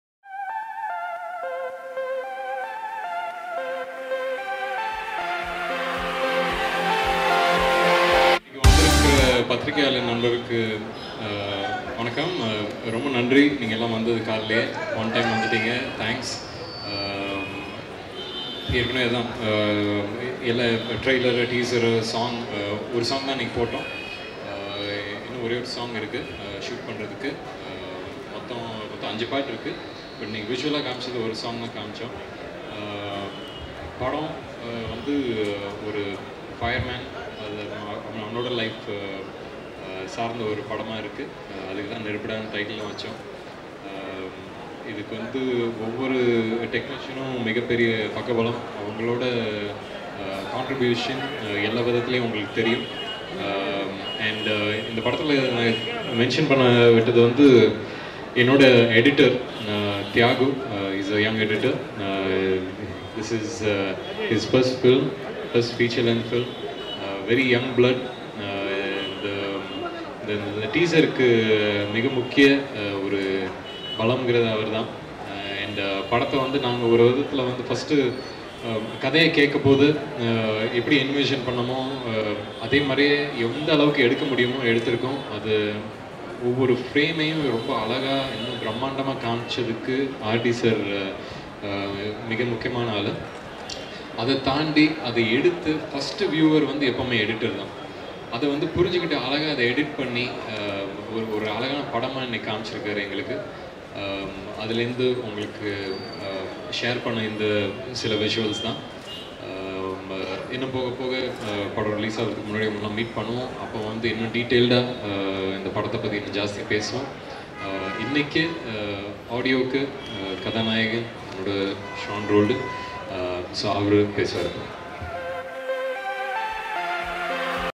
Vikram Prabhu: Editor Thiyagu is the biggest strength for Neruppu Da Teaser
Neruppu Da press meet was held in Chennai on April 10. Prabhu, Vikram Prabhu, Nikki Galrani, Music composer Sean Roldan, director Ashok Kumar and the rest of the cast and crew attended the event. Vikram Prabhu, the hero of the film praised editor Thiyagu for his brilliant work for the film’s teaser. Editor Thiyagu is making his debut with this film.